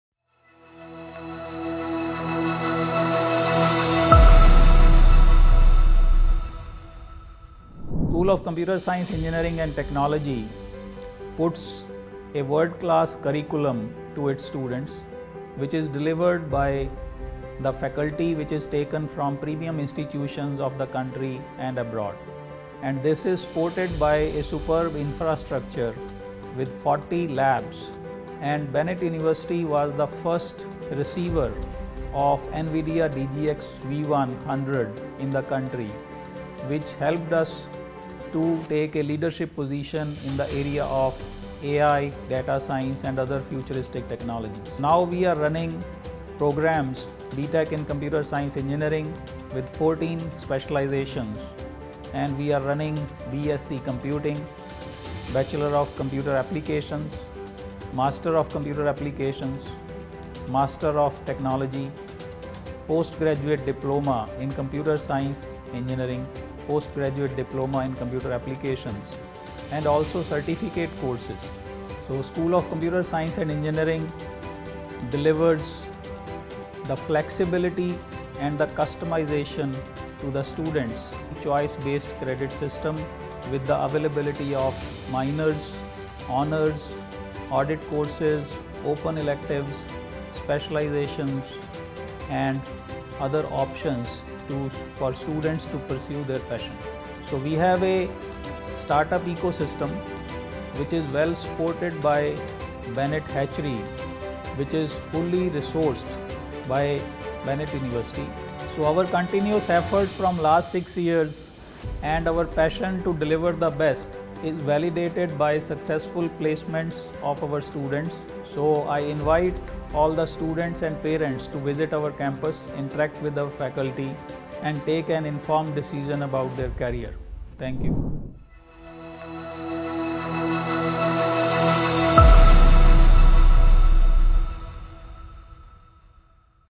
The elevator to success by- Dr. Deepak Garg (Dean, School of Computer Science Engineering and Tech.)
Dr. Deepak Garg (Dean, School of Computer Science Engineering and Technology) is here to throw some light on the accomplishments of Bennett University. At the SchoolofComputerScienceEngineeringandTechnology, Bennett boasts of a world-classcurriculum delivered by top-notchfaculties from premium institutes. The campus offers an unmatchedinfrastructure with 40 well-equipped labs. Apart from this Bennett was the first private university to receive the supercomputer NVIDIADGXV100. The school offers 14 most sought-after specialisations and a choice-basedcreditsystem that provides students with the flexibility that they seek. The start-up culture on campus thrives on BennettHatchery, fully supported by BU. The highest placements packages of up to ₹ 57 LPA and an average of ₹ 11 LPA are the proof of the excellence it strives to deliver each moment.